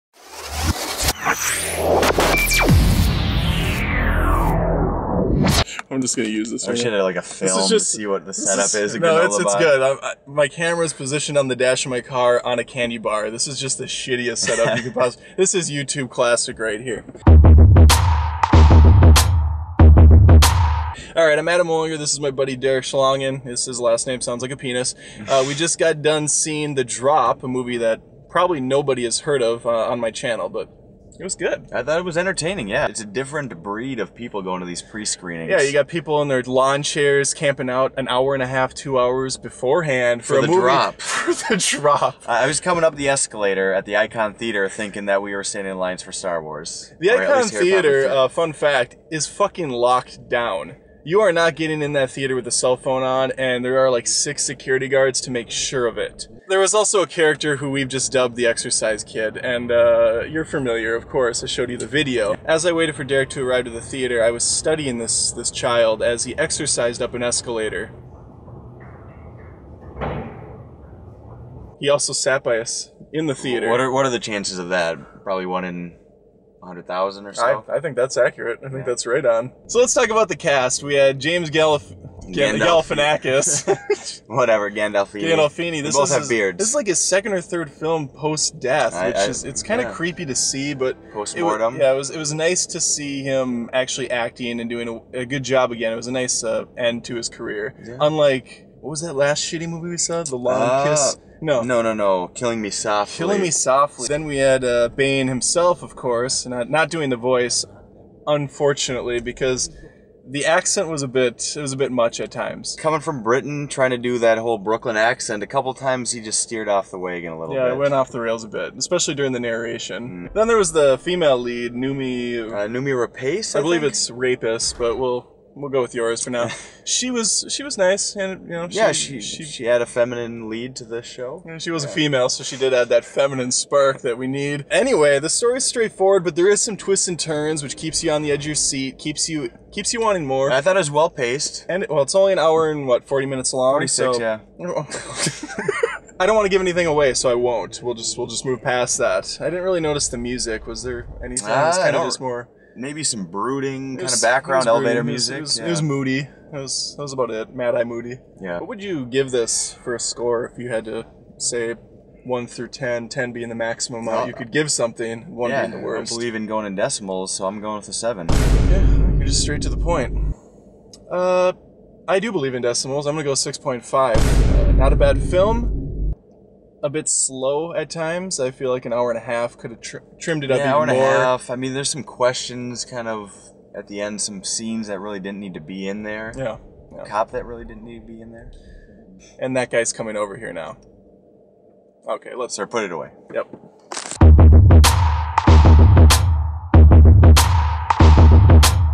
The Drop Movie Review : Carside Reviews ep1
This is the first episode of Carside Reviews. A new show on Movie Feuds where Adam gives his fresh take on a film just after viewing.
The Drop is the first movie to get the Carside treatment. Hope you guys and enjoy.

The Drop Synopsis via IMDB:
Bob Saginowski finds himself at the center of a robbery gone awry and entwined in an investigation that digs deep into the neighborhood's past where friends, families, and foes all work together to make a living - no matter the cost.